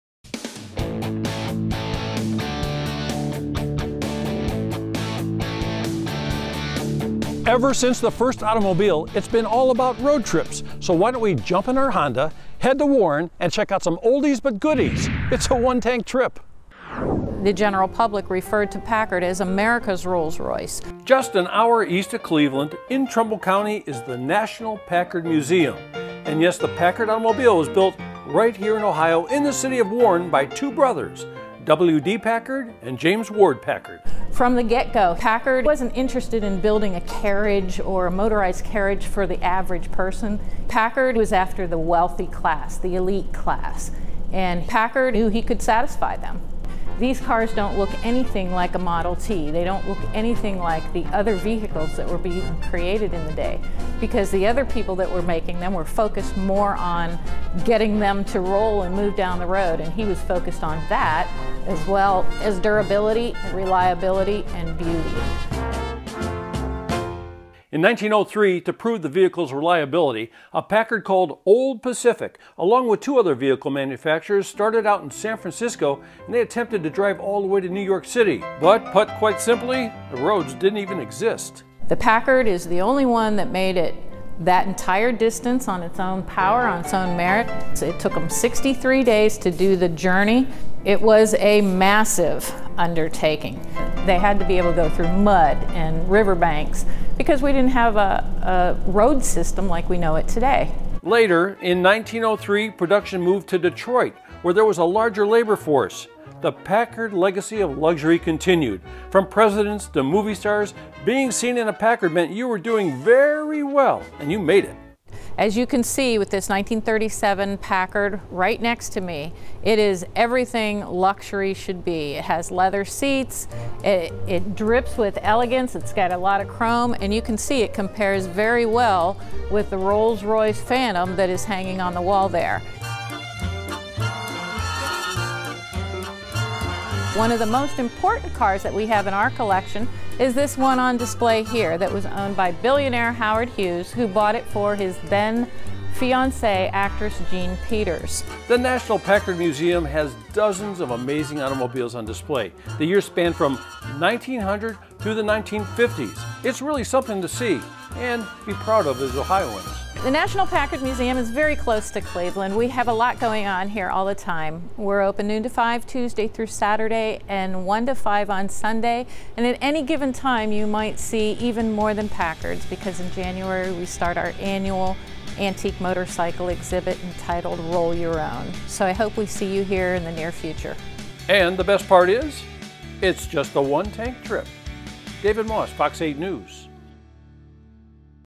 OTT Packard pkg
David Moss visits the "National Packard Museum" in Warren Ohio.